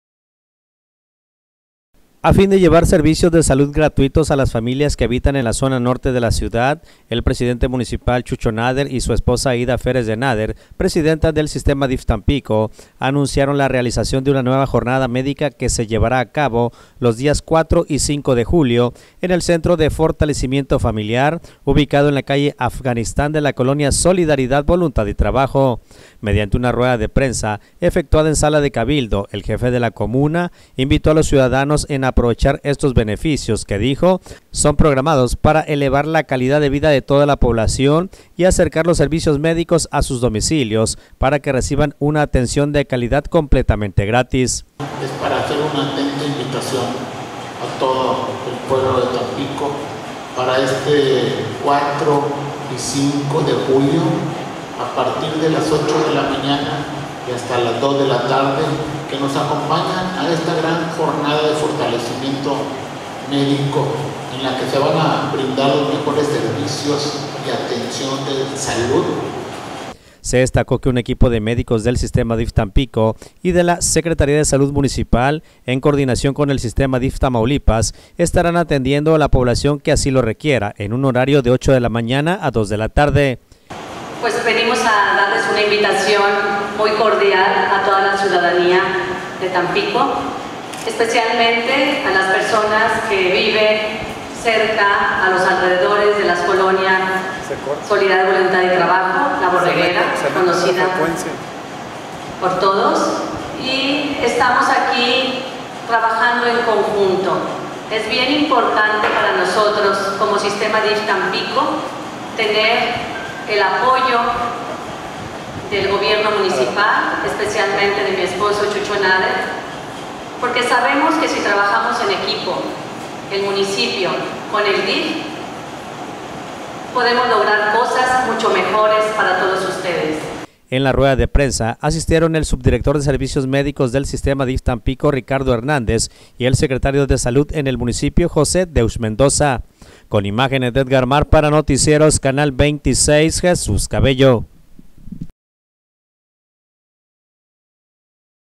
SE LLEVARA A CABO JORNADA MEDICA EN TAMPICO.
A fin de llevar servicios de salud gratuitos a las familias que habitan en la zona norte de la ciudad, el Presidente Municipal Chucho Nader y su esposa, Aida Feres de Nader,  Presidenta del Sistema DIF Tampico, anunciaron la realización de una nueva Jornada Médica que se llevará cabo los días 4 y 5 de julio en el Centro de Fortalecimiento Familiar, ubicado en la calle Afganistán de la colonia Solidaridad, Voluntad y Trabajo.